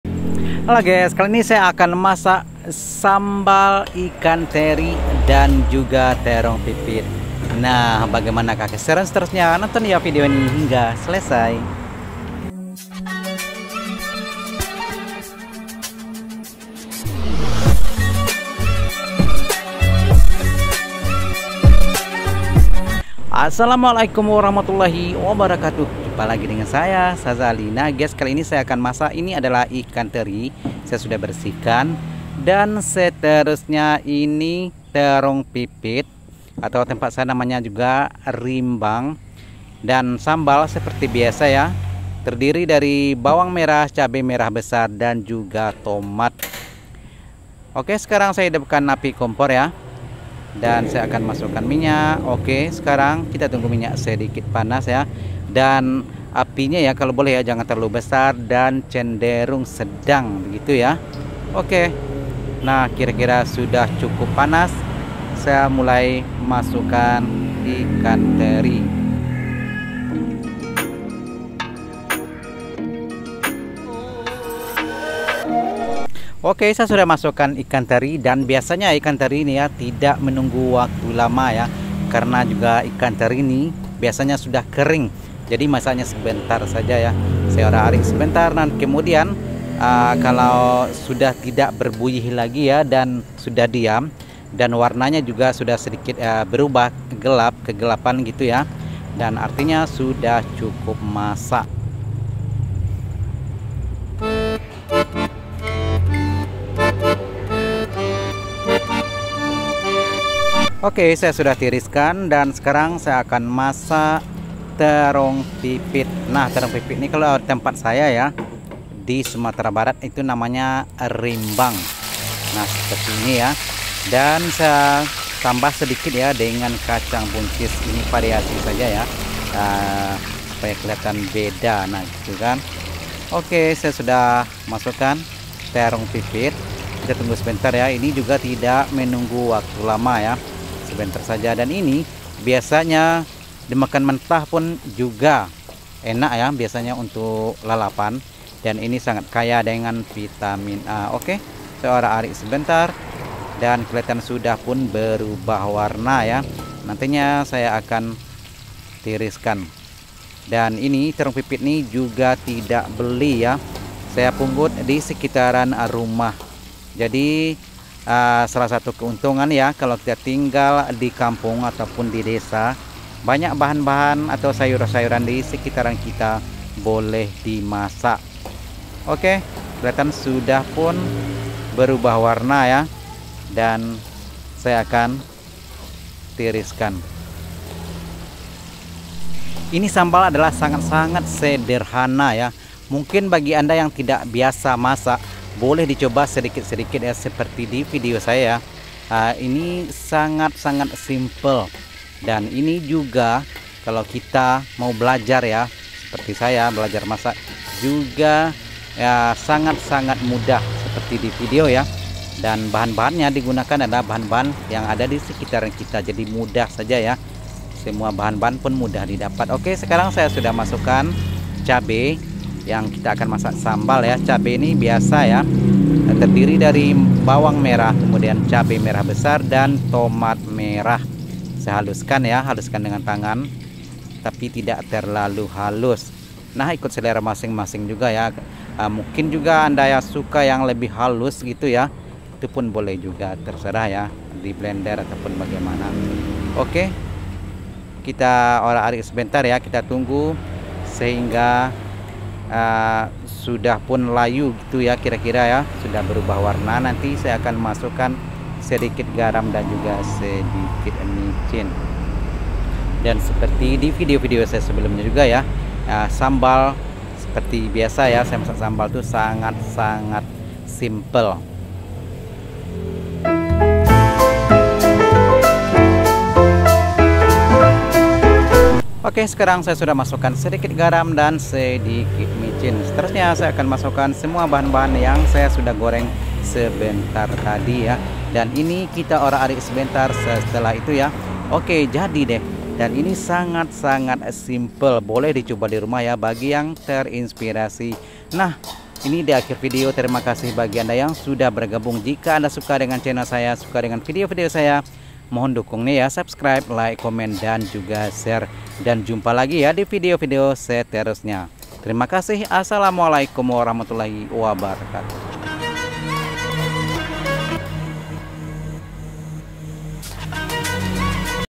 masak sederhana enak murah meriah,sambal teri terong Pipit @Shahzalikuliner
Nah guys menu saya hari ini,Masi masakan kampung, masakan sederhana, masakan Minang yaitu sambal ikan teri dan rimbang/terong Pipit, boleh di coba ya..